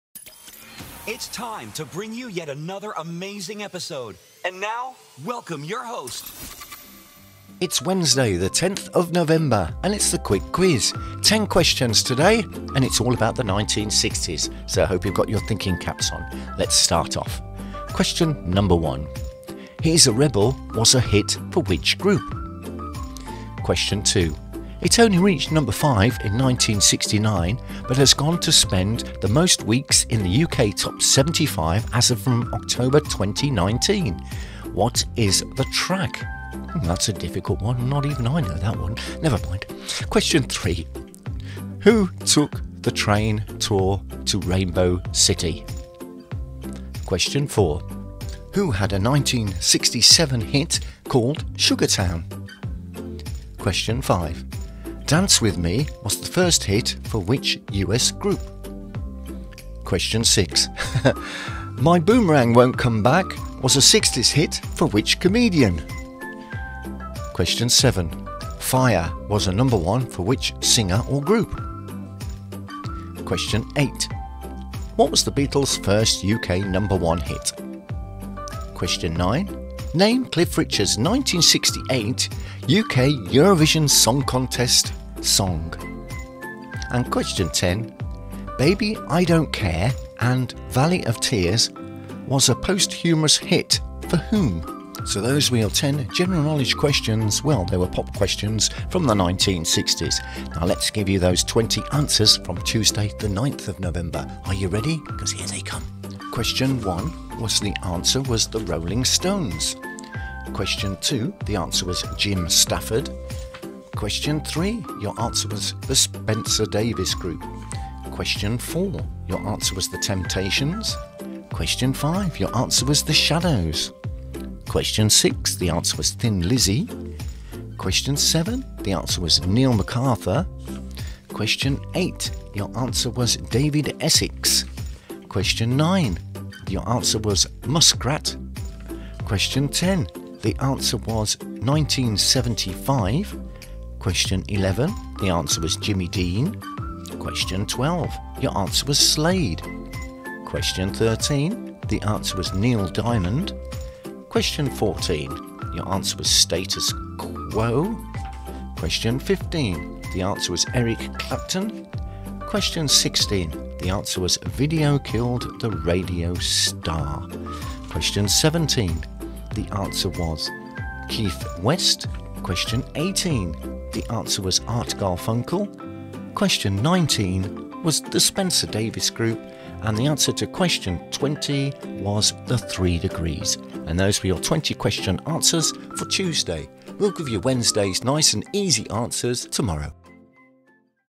The Quick Quiz November 10th 2021
The Quick Daily Quiz - Now Every Day
A Quick Quiz Usually under 4mins To Keep You Interested
General Knowledge Questions 1-15
Now Every Day